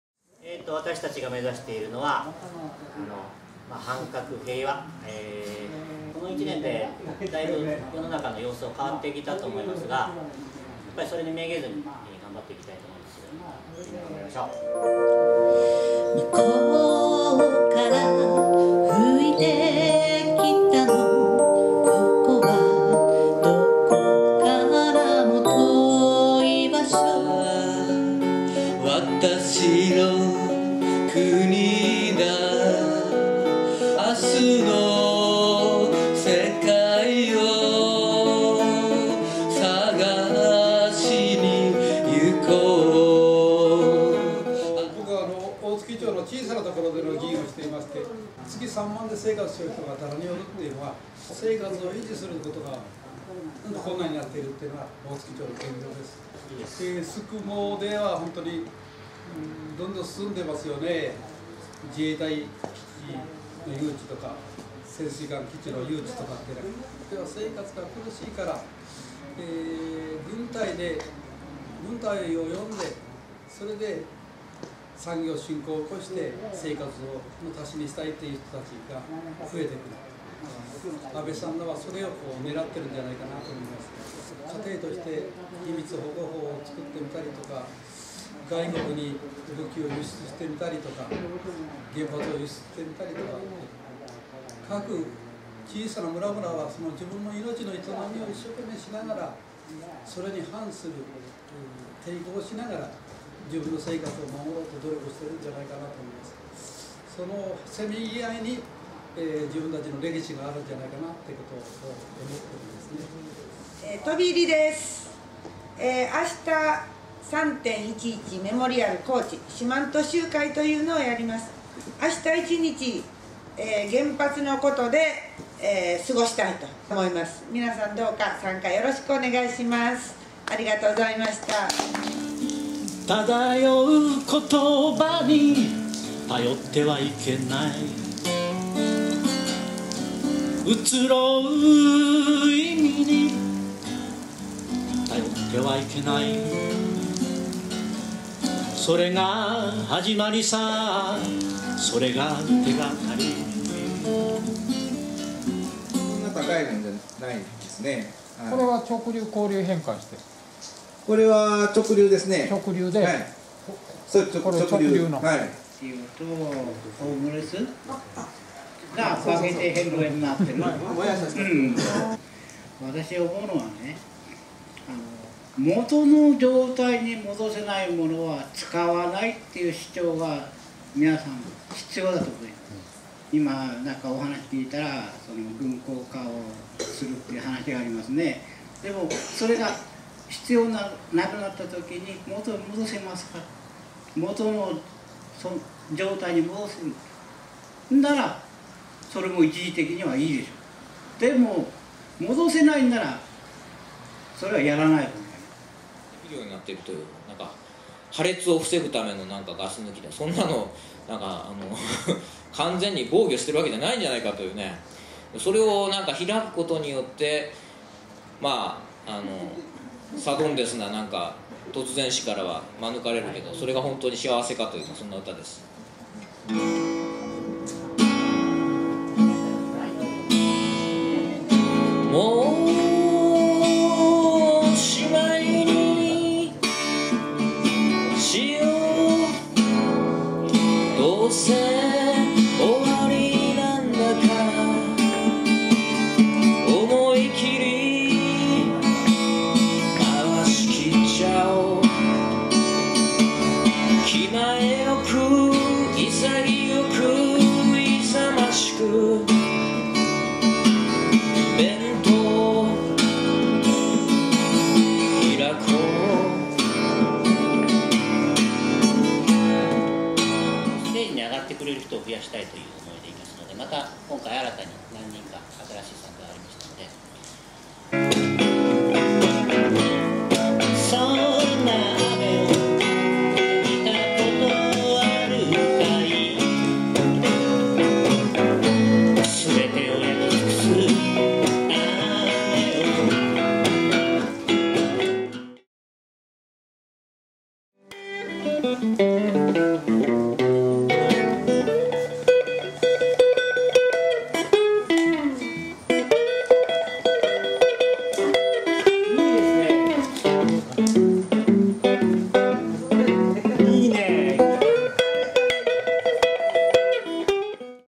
第3回宿毛ピース・ライヴ「春一番］ダイジェスト
2015/3/7宿毛サニーサイドパーク野外ステージにて第3回宿毛ピース・ライヴ「春一番］を開催した。雨のため観客席をステージ上に設置し、新たな出演者を迎え少人数ながらも密度の濃いライヴとなった。